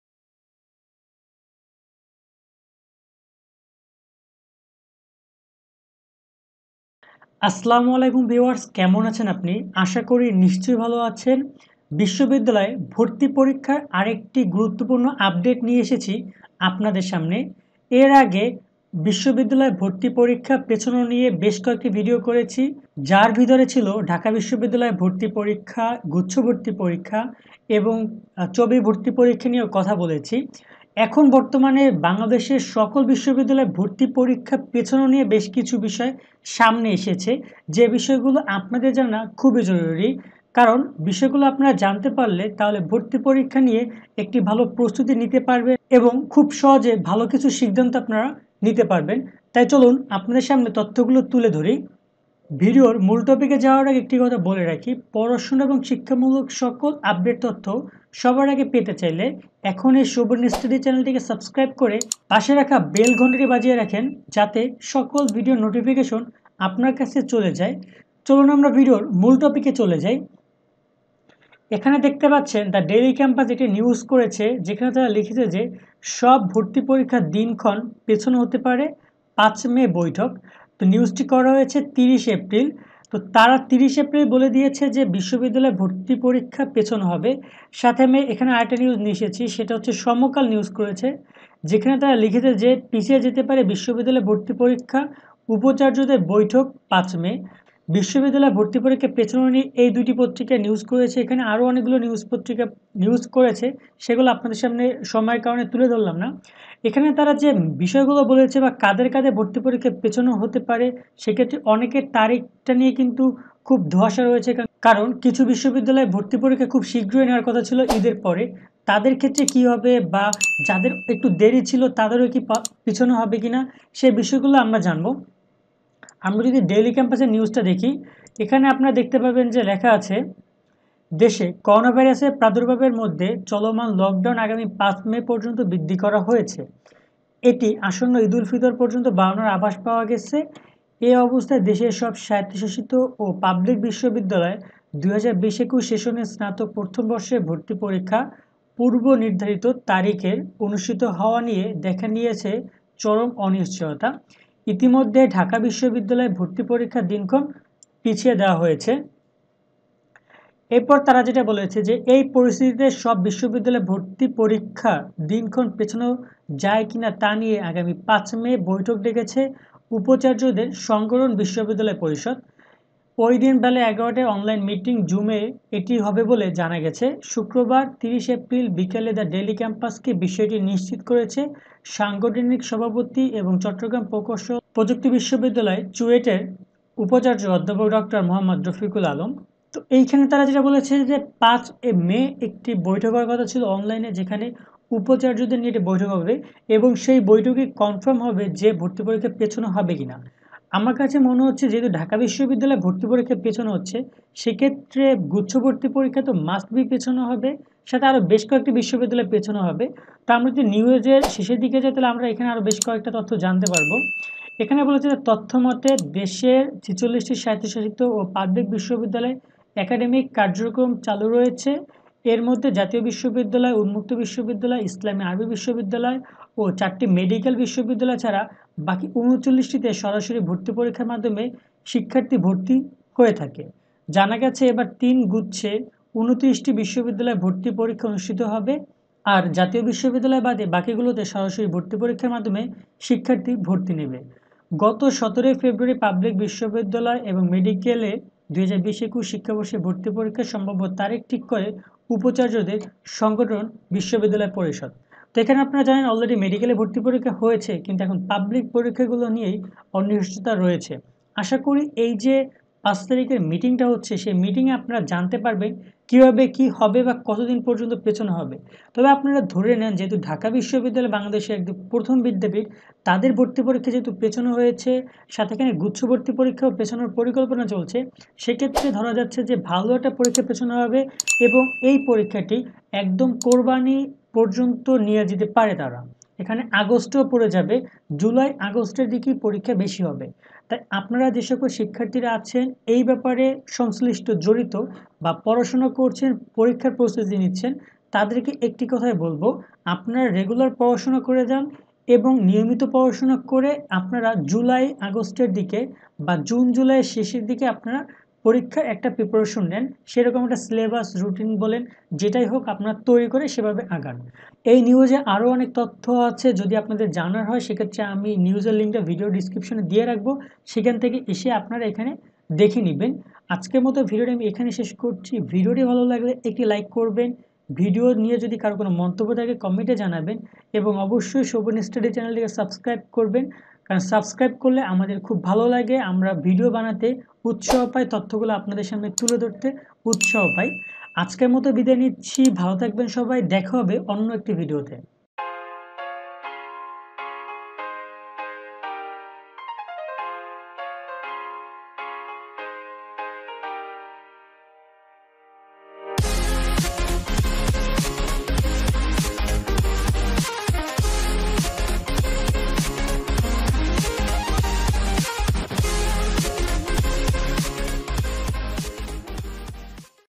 All University admission exam 2021, Guccho vorti porikkha 2021, সকল বিশ্ববিদ্যালয় ভর্তি পরিক্ষা ২০২১
দেশে করোনাভাইরাসের প্রাদুর্ভাবের মধ্যে চলমান লকডাউন আগামী ৫ মে পর্যন্ত বৃদ্ধি করা হয়েছে। এটি আসন্ন ঈদুল ফিতর পর্যন্ত বাড়ানোর আভাস পাওয়া গেছে। এ অবস্থায় দেশের সব স্বায়ত্তশাসিত ও পাবলিক বিশ্ববিদ্যালয়ের ২০২০-২১ সেশনের স্নাতক প্রথম বর্ষের ভর্তি পরীক্ষা পূর্বনির্ধারিত তারিখে অনুষ্ঠিত হওয়া নিয়ে দেখা দিয়েছে চরম অনিশ্চয়তা। ইতোমধ্যে ঢাকা বিশ্ববিদ্যালয়ের ভর্তি পরীক্ষার দিনক্ষণ পিছিয়ে দেওয়া হয়েছে।

এই পরিস্থিতিতে সব বিশ্ববিদ্যালয়ের ভর্তি পরীক্ষার দিনক্ষণ পেছানো যায় কিনা তা নিয়ে আগামী ৫ মে বৈঠক ডেকেছে উপাচার্যদের সংগঠন বিশ্ববিদ্যালয় পরিষদ। ওইদিন বেলা ১১টায় অনলাইন মিটিং জুমে এটি হবে বলে জানা গেছে। আজ শুক্রবার (৩০ এপ্রিল) বিকেলে দ্যা ডেইলি ক্যাম্পাসকে বিষয়টি নিশ্চিত করেছেন সংগঠনটির সভাপতি এবং চট্টগ্রাম প্রকৌশল ও প্রযুক্তি বিশ্ববিদ্যালয়ের (চুয়েট) উপাচার্য অধ্যাপক ড. মোহাম্মদ রফিকুল আলম।

🔴 মাধ্যমিক ও উচ্চ মাধ্যমিকঃ